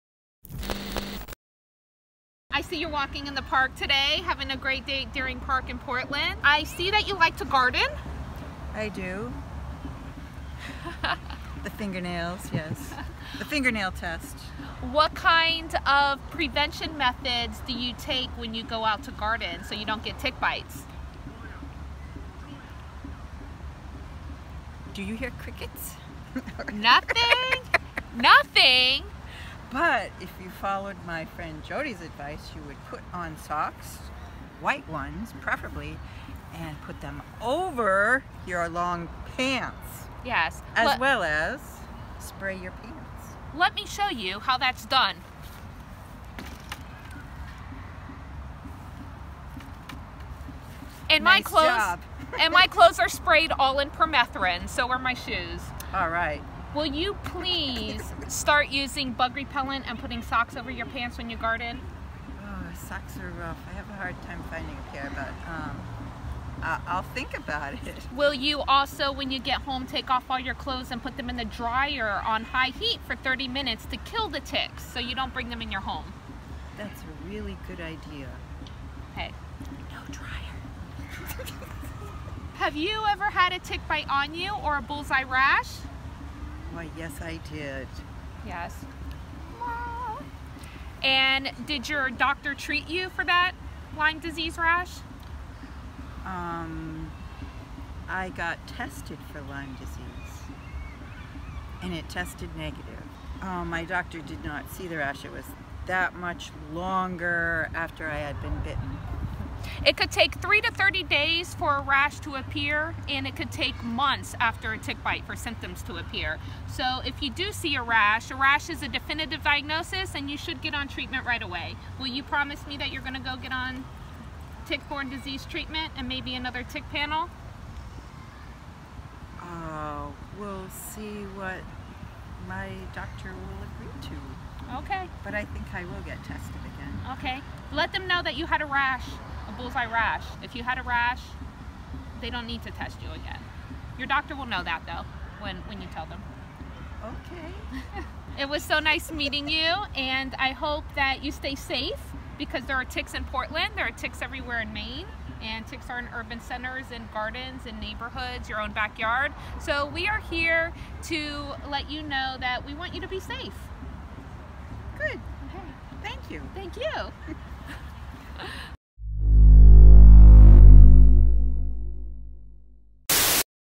Street Beatz Random People Episode #1 | LymeTV
Filmed in the summer of 2019 & never-before published on YouTube.

LymeTV hit the street for our Street Beatz interviews at Deering Oaks Park in portlandmaine where we randomly asked people questions about tick bite prevention & safety. In this episode, we speak with the awesome & entertaining Bonnie Bump about Lyme disease.

to get the facts about dangerous tick disease prevention.